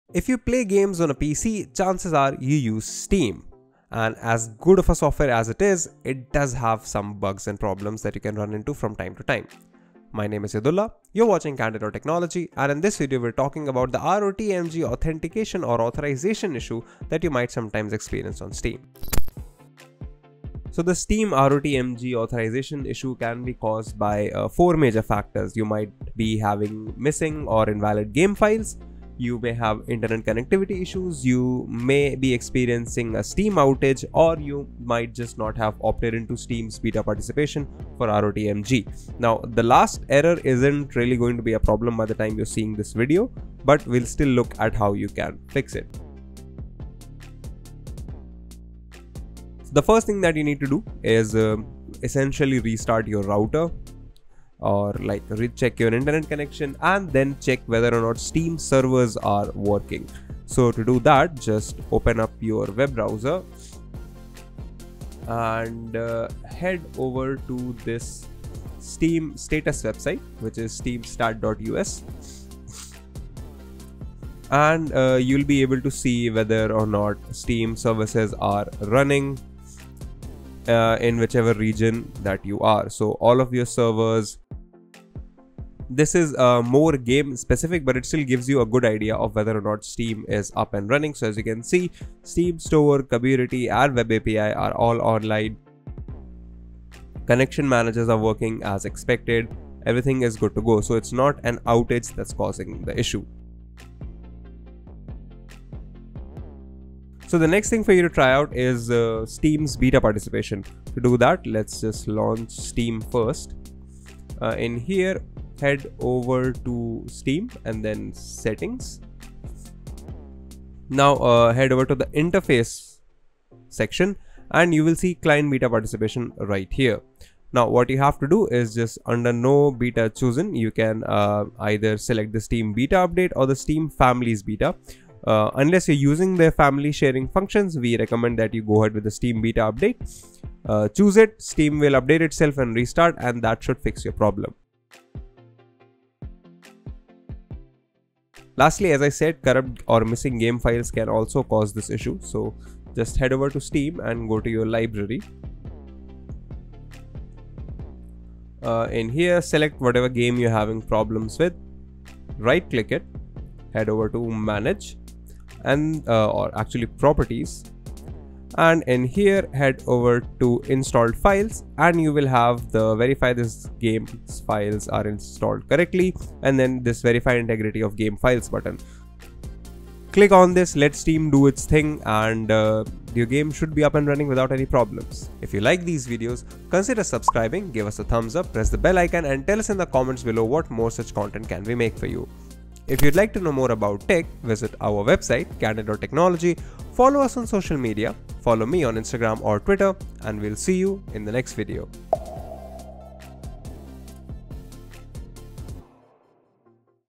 Fix: Rotmg Steam Authorization Error | Candid.Technology
Here is how to fix Rotmg Steam authorization error. for more guides and tech explainers, reviews, lists, comparisons and more tech-related stuff.